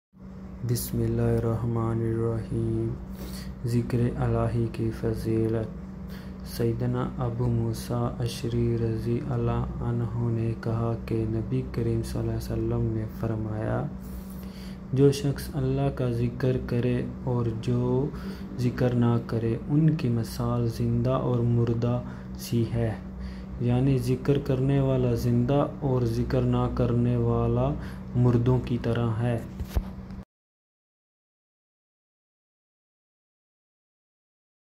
Zikr-E-Elahi Ki Fazeelat || ذکر الٰہی کی فضیلت || Hadees-e-Nabwi ﷺ
Ye Hadess ..Sahih Bukhari Se Lee Gai Hain..Jis Me Nabi Pak ﷺ NA Farmaya Hai Zikar Karna Wala Zinda Aur Zikar Na Karna Murdo Ke Tara Hain.

In Malaysia  Language 🇲🇾
Keutamaan mengingati Allah.

   Sayyidna Abu Musa Al-Asy'ari (RA) berkata bahawa Nabi (SAW) bersabda, "Sesiapa yang menyebut Allah dan sesiapa yang tidak menyebutnya adalah seperti orang yang hidup dan yang mati."  (iaitu orang yang ingat hidup dan orang yang tidak ingat adalah seperti orang mati)

   Sahih Bukhari

In English  Language

Virtue of remembrance of God.

   Sayyidna Abu Musa Al-Ash'ari (RA) said that the Prophet (PBUH) said, "Whoever mentions Allah and whoever does not mention him are like the living and the dead."  (i.e. the one who remembers is alive and the one who does not remember is like the dead)

   Sahih Bukhari